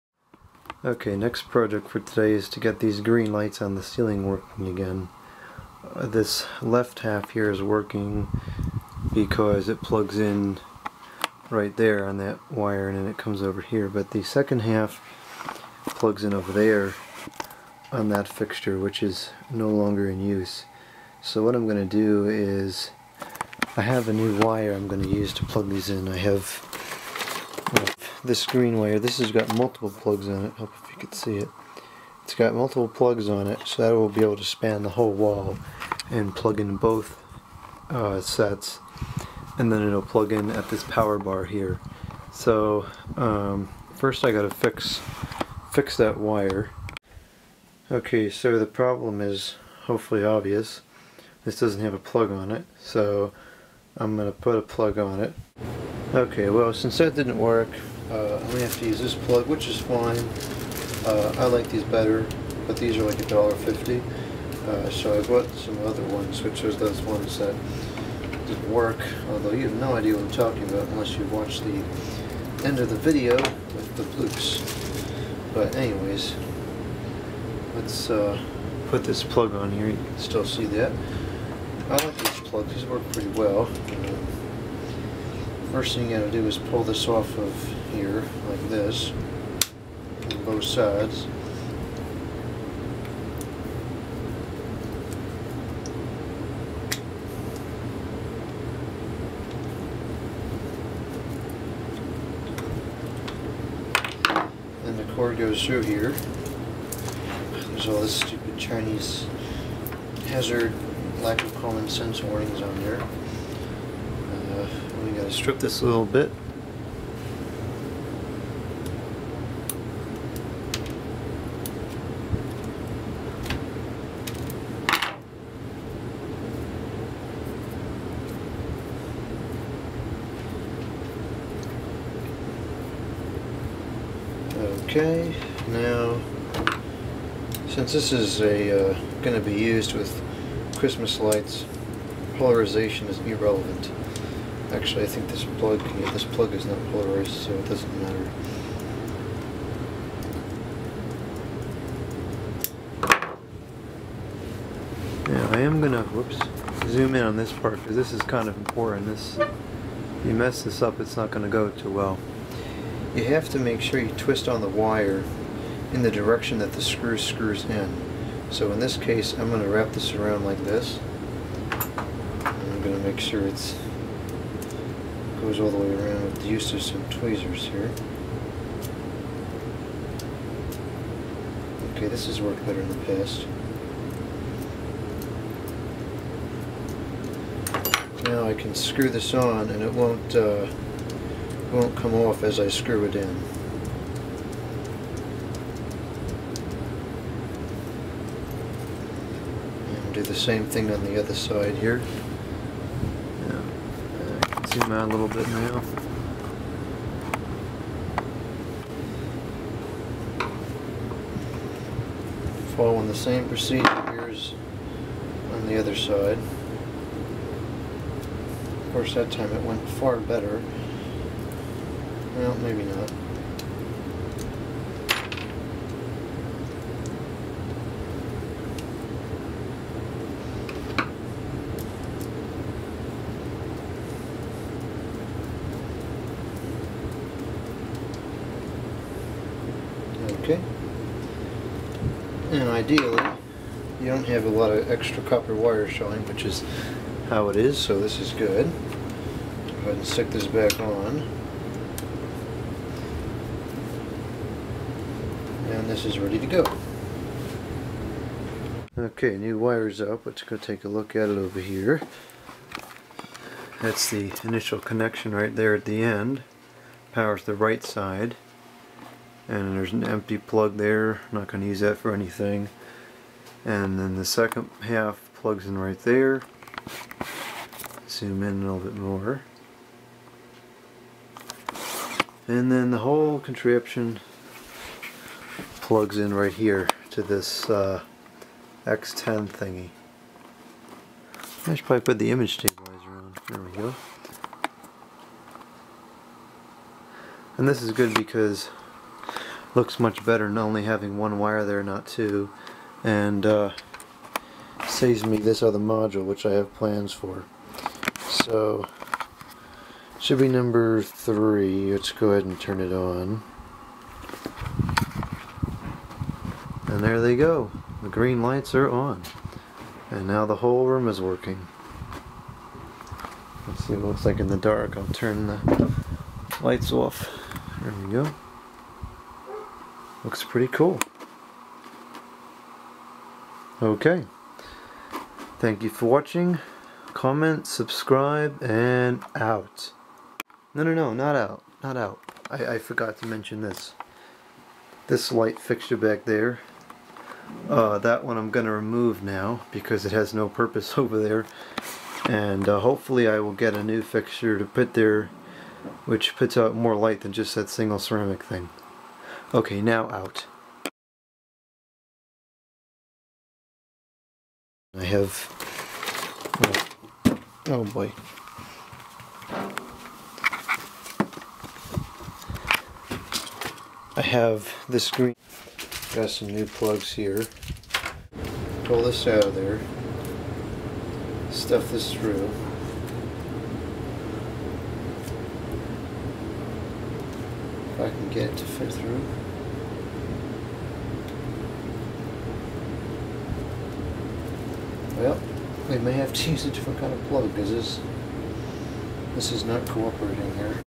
Install New Wire for Green Lights in the Fan Room
Installing a new plug and hanging the wire for the green ceiling lights in the fan room..